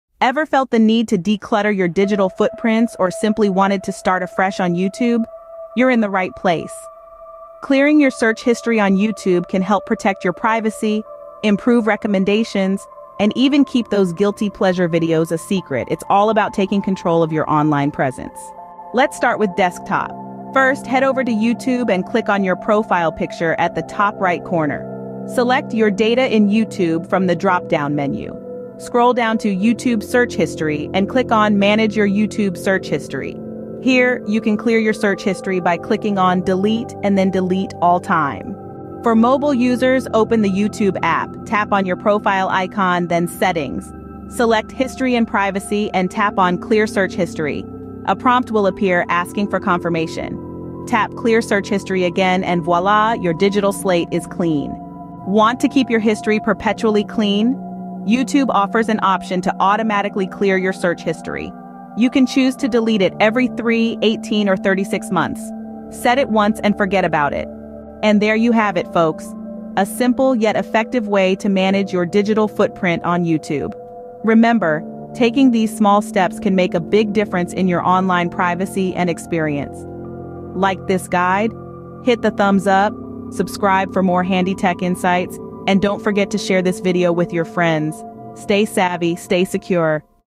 Clear Your Tracks A Step by Step Guide to Erasing Your YouTube Search History
"Clear Your Tracks: A Step-by-Step Guide to Erasing Your YouTube Search History Revealed!"

Are you worried about your YouTube search history? 🤔 In this video, we unveil the ultimate step-by-step guide to erasing your YouTube search history for good! Learn how to keep your browsing private and secure. Watch now and take control of your online footprint! 🔍🔒